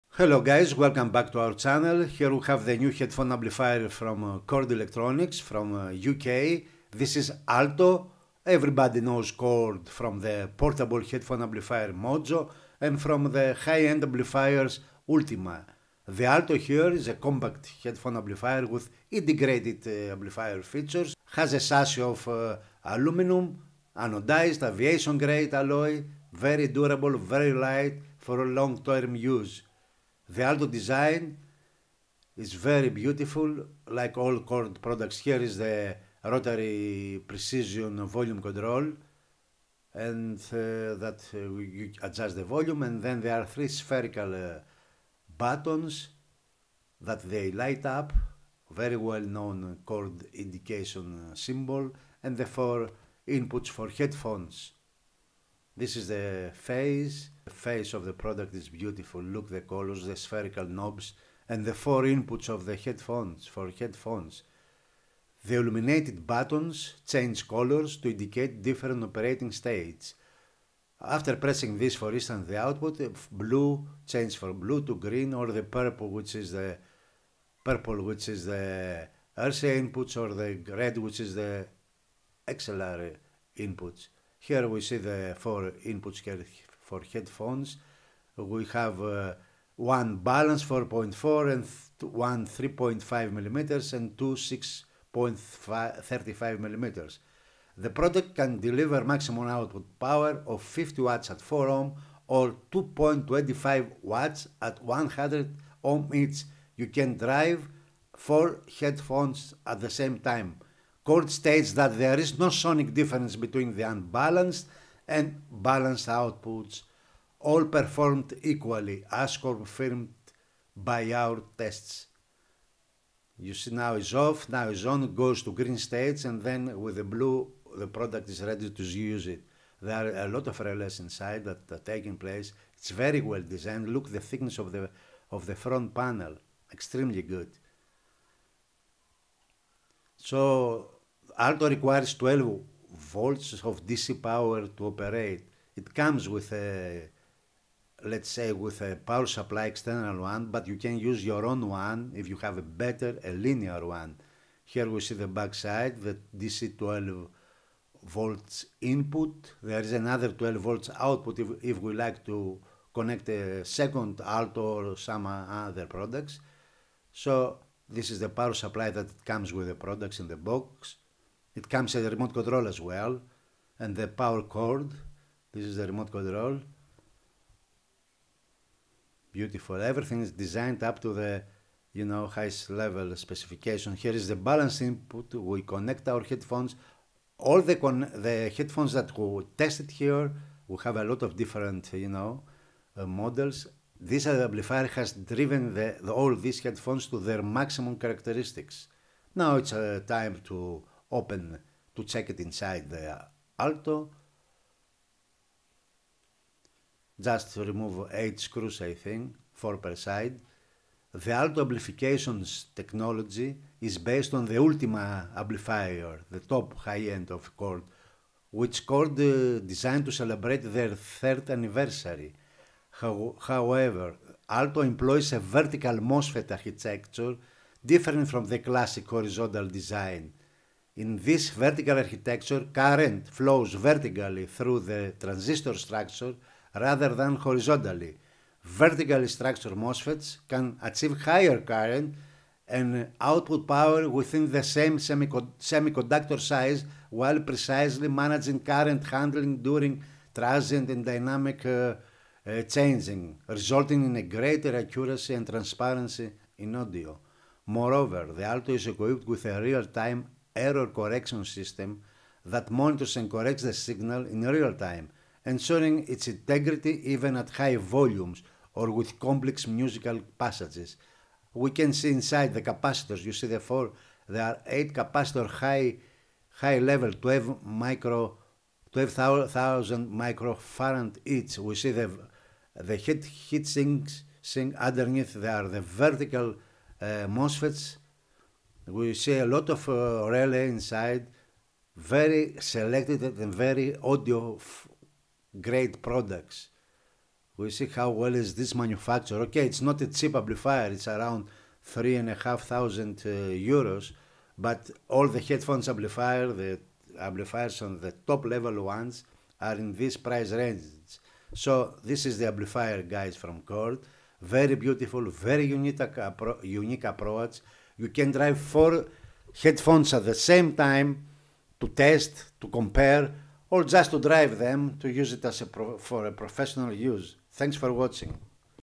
High-Fidelity Sound! Chord ALTO Headphone Amp
We know Chord very well—not only for their portable headphone amplifier, Mojo, which has been prominent in the market for years, but also for their top-tier Ultima amplifiers frequently showcased at high-end shows.

The Alto headphone amplifier borrows its amplification stage from the Ultima series, which prompted us to bring it in for testing in our magazine "hxosplus".

The Alto is a compact headphone amplifier (with built-in an integrated amplifier). Its chassis is crafted from anodized aviation-grade aluminum alloy, making it lightweight and durable for long-term use.

The Alto's design inherits the family styling of other Chord products.

On the left side of its front panel, it features a high-precision rotary volume knob and three spherical buttons (known as the colorful beads): a large on/off button, a smaller input selection button (on the left), and another (on the right) to toggle output.

The illuminated buttons change colors to indicate different operating states. For instance, after pressing the central button on the front panel, its light changes from red to green, indicating that the device is now active. Pressing the left button lets you select XLR input (red) or RCA input (purple).

The right button toggles between headphones (green), speakers (red), or line-out (blue). The device's operation logic is simple and intuitive, and it even includes a remote control.

On the right side of the front panel, there are four headphone jacks: one 4.4mm balanced, one 3.5mm, and two 6.35mm jacks.

These can simultaneously drive up to four pairs of headphones, delivering maximum output power of 50W/4Ω (or 2.25W/100Ω) to each.

Chord states that there is no sonic difference between the unbalanced and balanced outputs—all perform equally, as confirmed by our tests. At the back, the Alto features RCA and XLR inputs, as well as 4mm banana speaker terminals for speaker connections.

The Alto requires 12V DC to operate. You can use the supplied power pack or an optional linear power supply for improved current supply. Additionally, the device offers a 12V DC output to power auxiliary equipment.

The included switch-mode power supply has been meticulously designed by Chord to ensure high-speed signal response and adequate dynamic reserves.

The Alto’s amplification technology is based on the Ultima amplifier topology, launched by Chord to celebrate their 30th anniversary. However, the Alto employs a vertical MOSFET architecture, differing from the classic horizontal design.

In this vertical architecture, current flows vertically through the transistor’s structure rather than horizontally. Vertically structured MOSFETs can achieve higher current and output power within the same semiconductor size while precisely managing current handling during transient and dynamic changes, resulting in greater accuracy and transparency.

Moreover, the Alto is equipped with a real-time error-correction system that monitors and corrects the signal in real-time, ensuring its integrity even at high volumes or with complex musical passages.